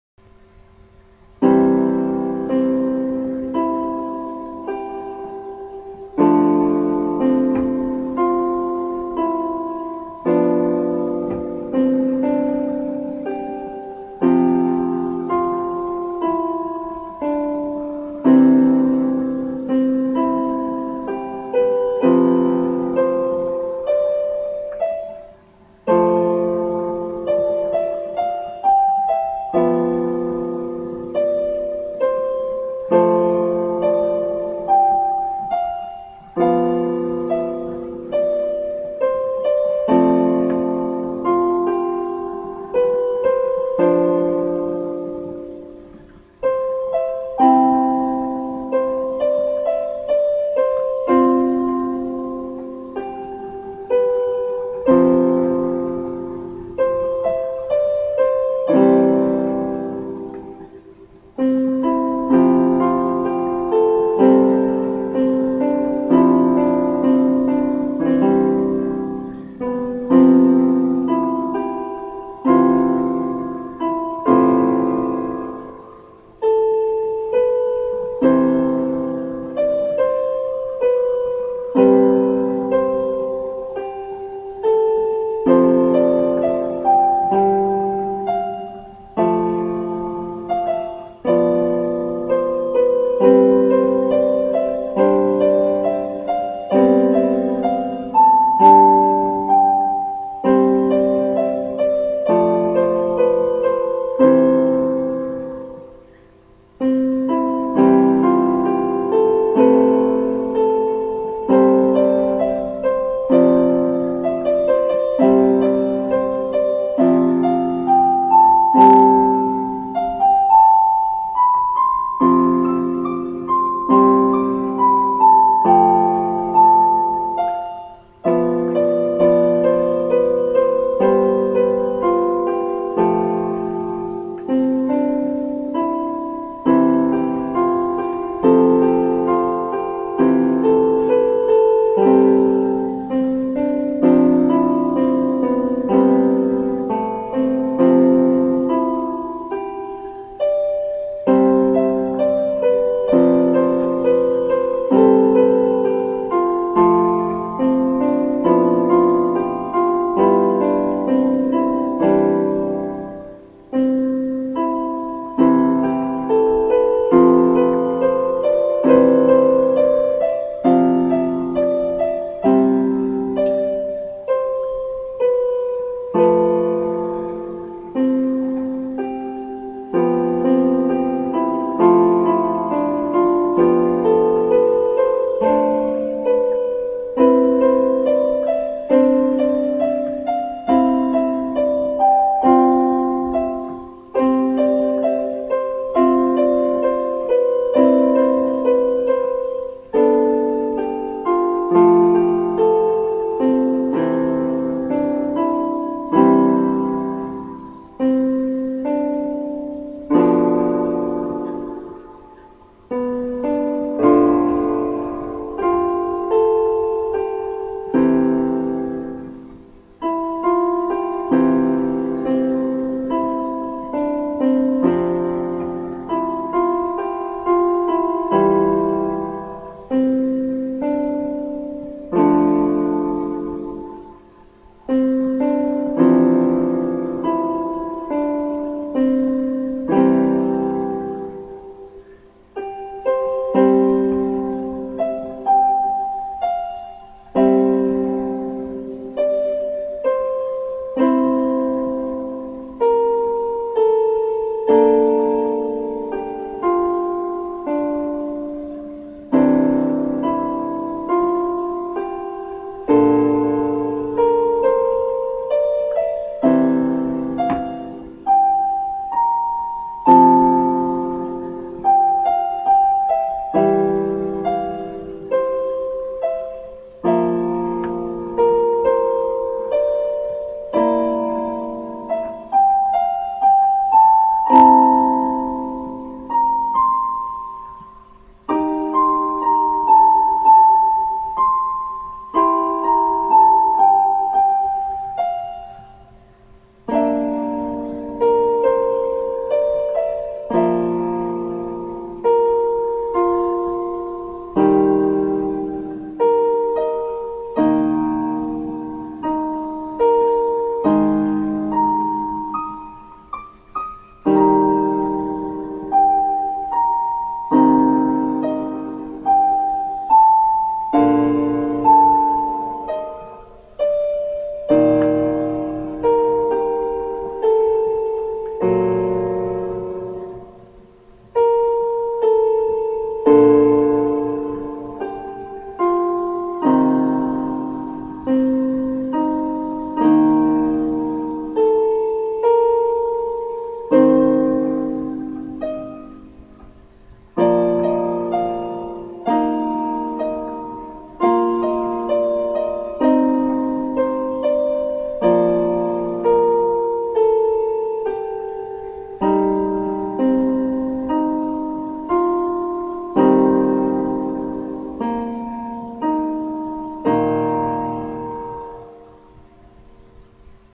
More piano music
This is just me messing around with different keys.